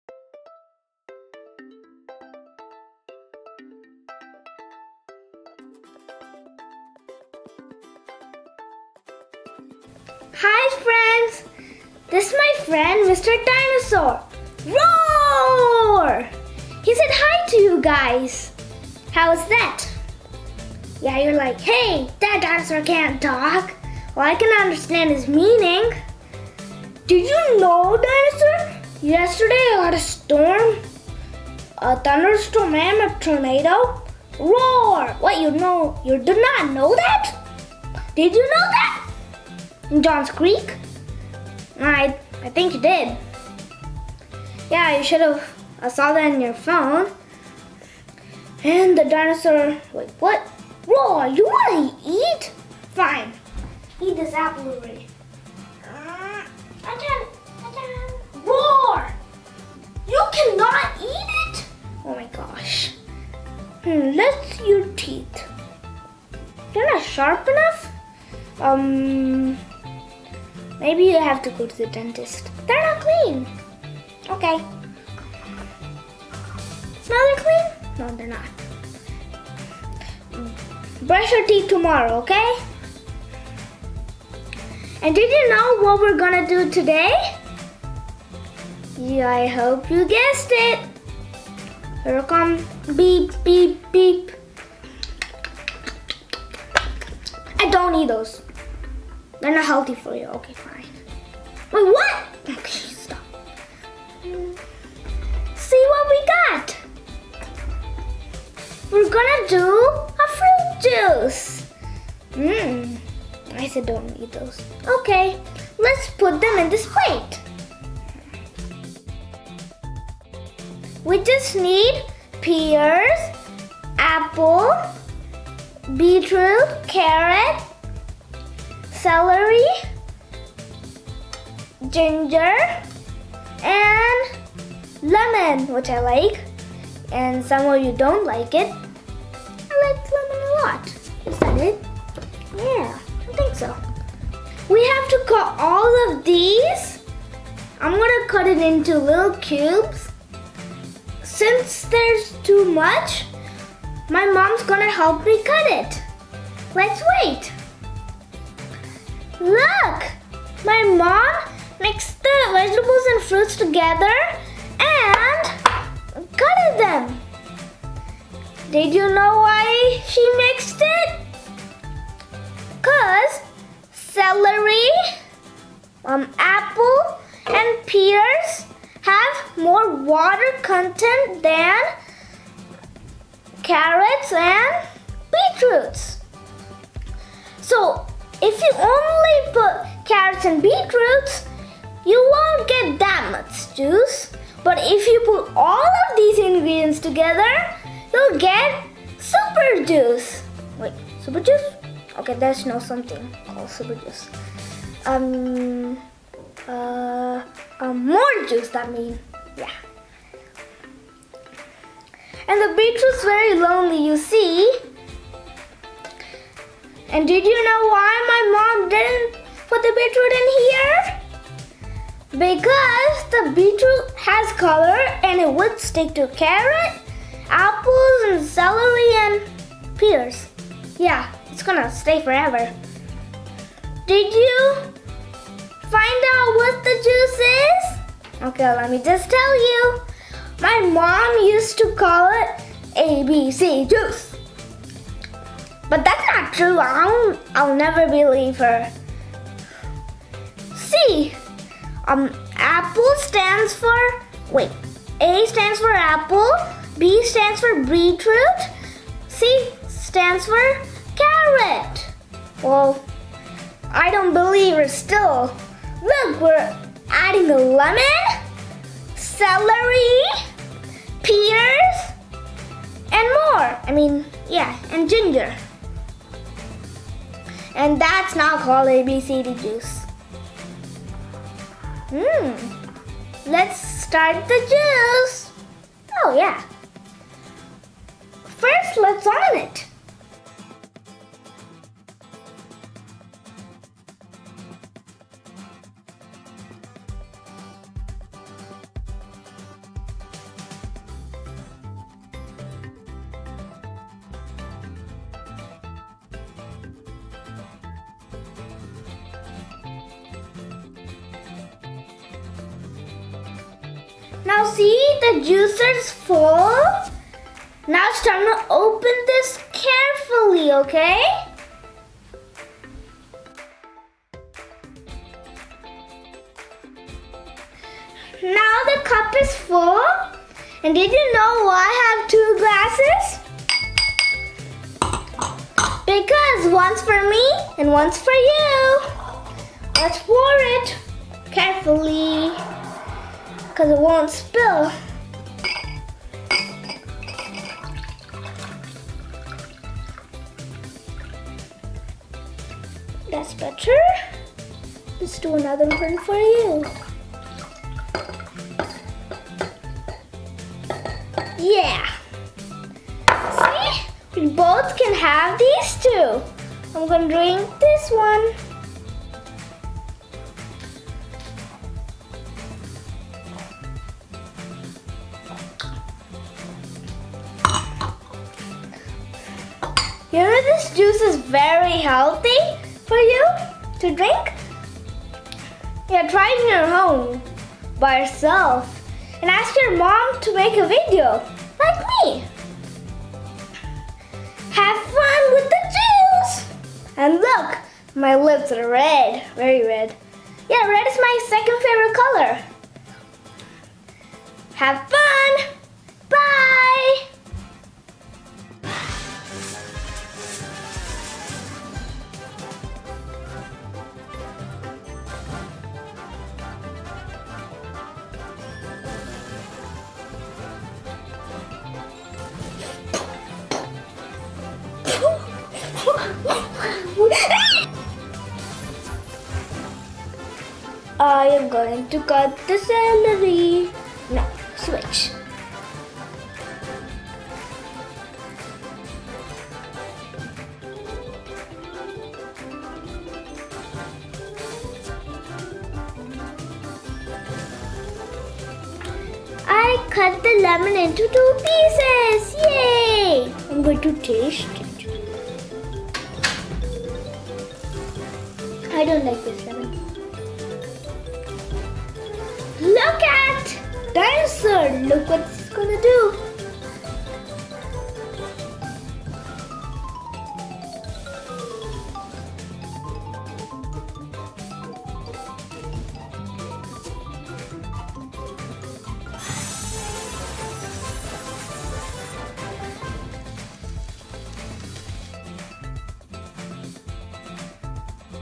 ABC Juice || How to make Mix Fruits and Vegetables Juice
Healthy Juice Recipes || How to make your own juice with fruits and vegetables || Simple and Easy  way to make ABC juice